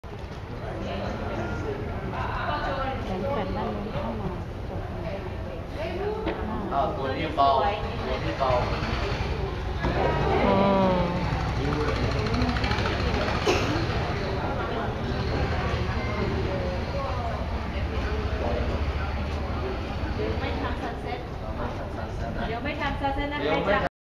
Nga Phe Kyaung Monastery (Jumping Cat Monastery) in Inle Lake, Myanmar
Monks at Nga Phe Kyaung Monastery in Inle Lake, Myanmar have trained cats to jump through hoops.